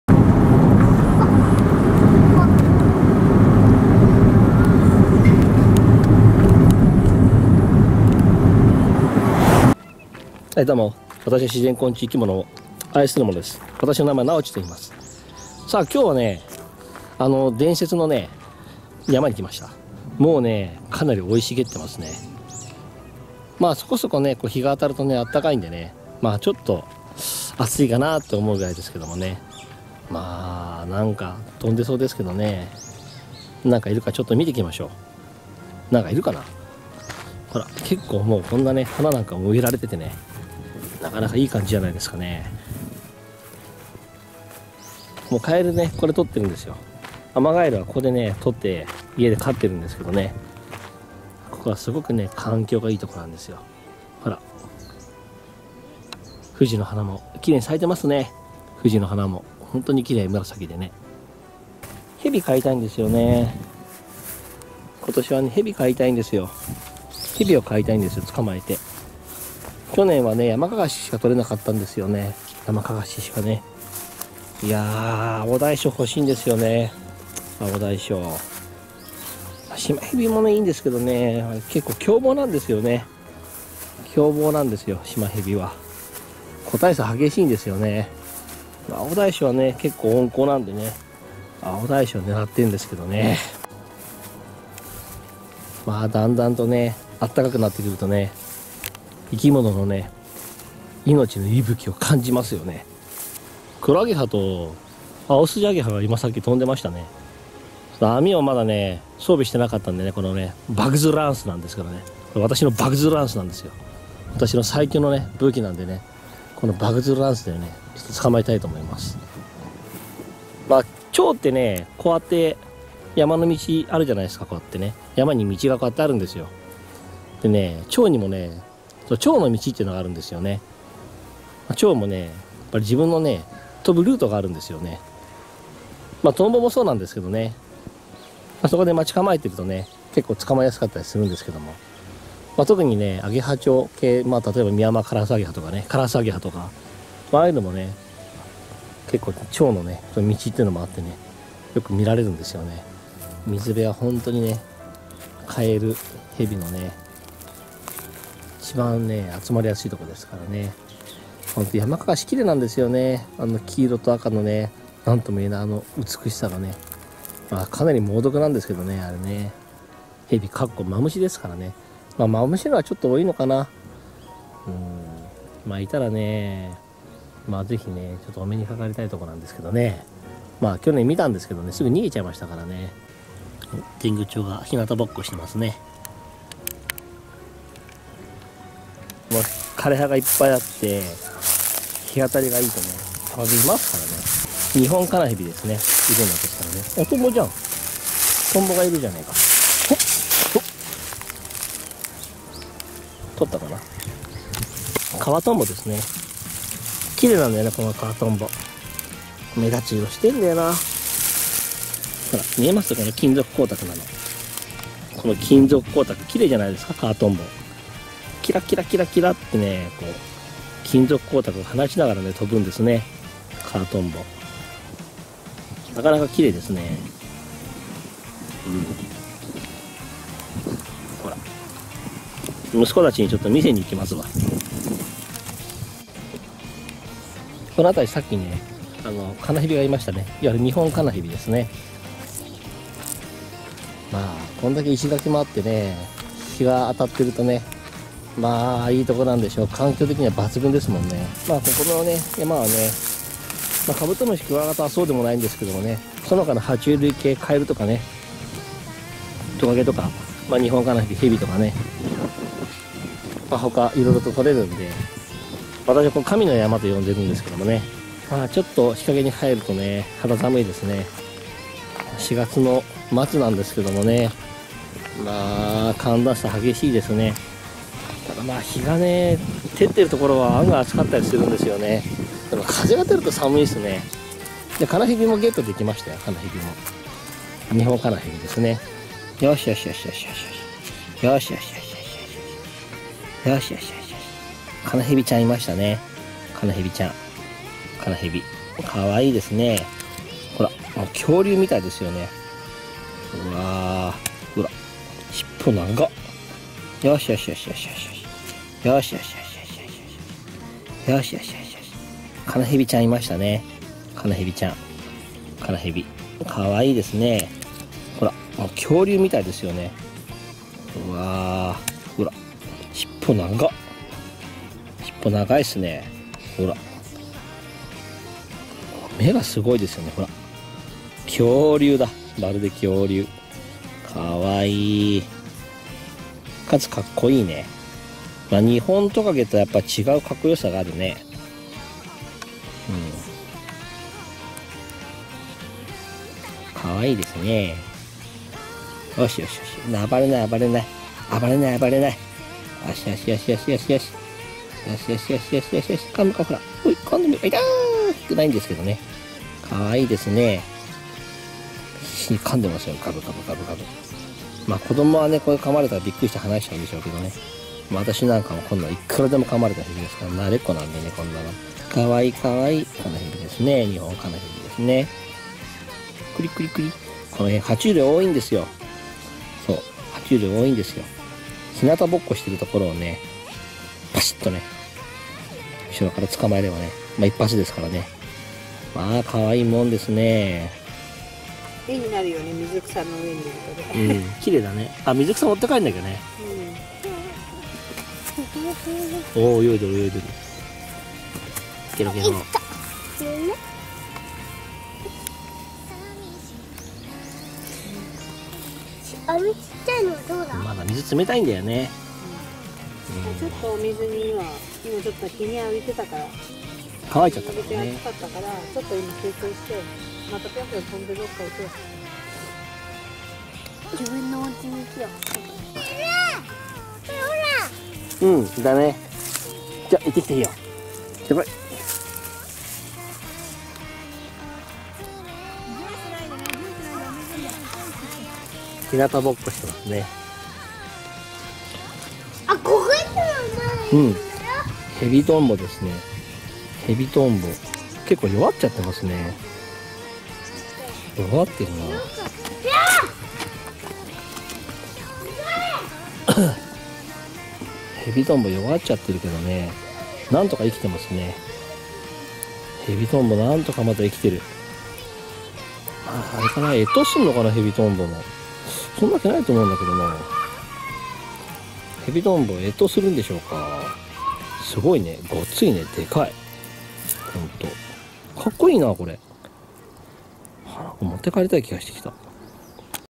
恐竜のラプトルみたい！Like a raptor！
神の山【わたしがそう感じている山】に来ました。良い感じに緑々としている。生き物の息吹きを感じました。Like a raptor#トカゲ#爬虫類#カナヘビ